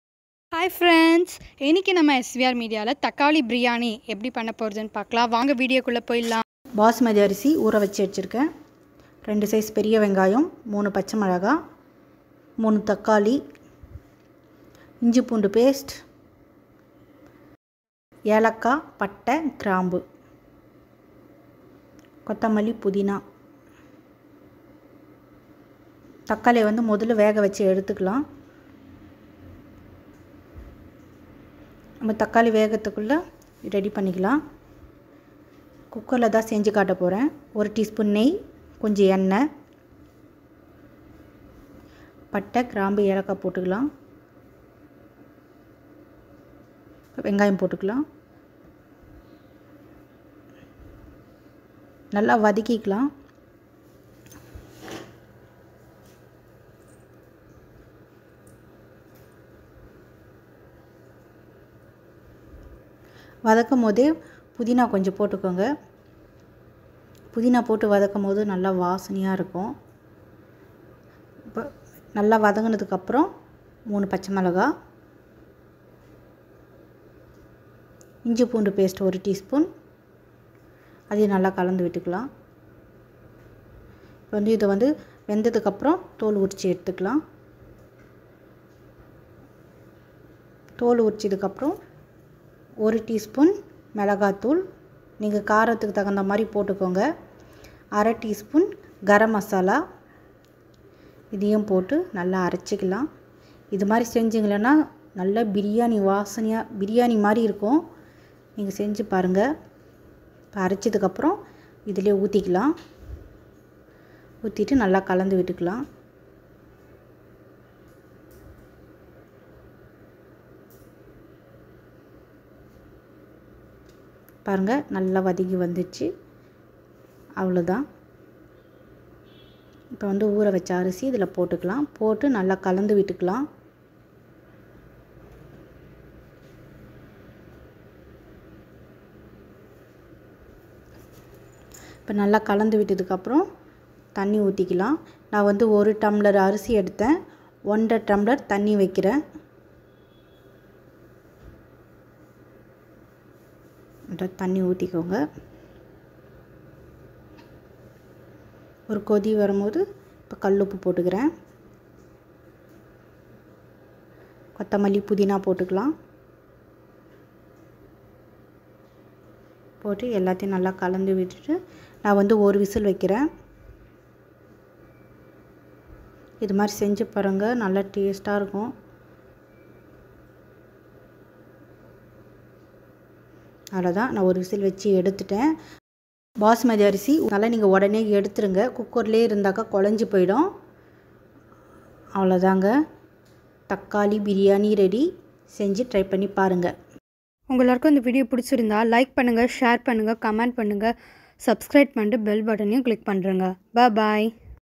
Tomato biryani | தக்காளி பிரியாணி
பிரியாணியை விட அதிக சுவையில் தக்காளி சாதம் செஞ்சு பாருங்க
தக்காளி பிரியாணி குக்கரில் குழையாமல் செய்யலாம்